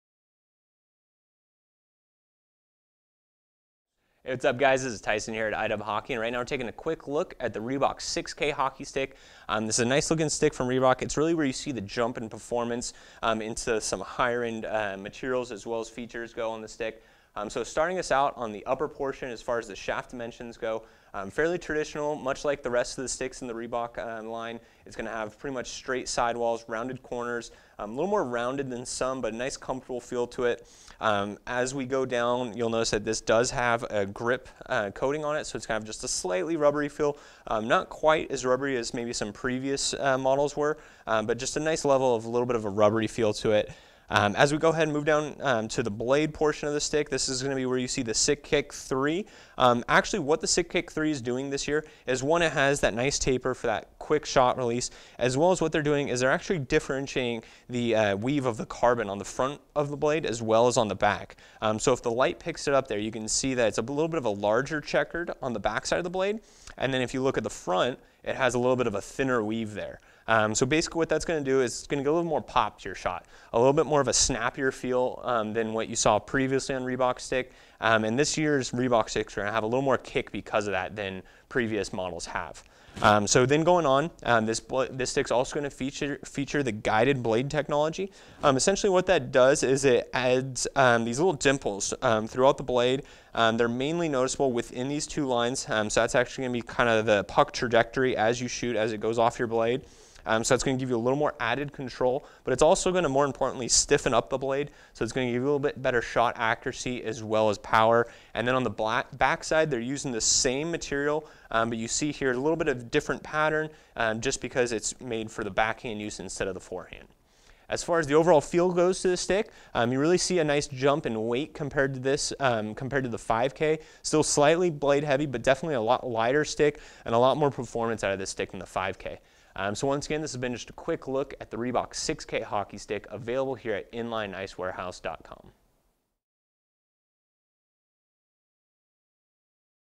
Reebok 6K Sickick III Grip Hockey Stick
The Reebok 6K Sickick III grip hockey sticks offers a great value with an affordable price and some high-end features. The 6K stick features the Sickick III technology which uses two different carbon weaves on the front and back of the stick to give an extra pop to the shot release.

The Reebok 6K hockey sticks features the guided blade technology which helps guide the puck on your shot release, and increases blade stiffness. Both helping increase shot accuracy.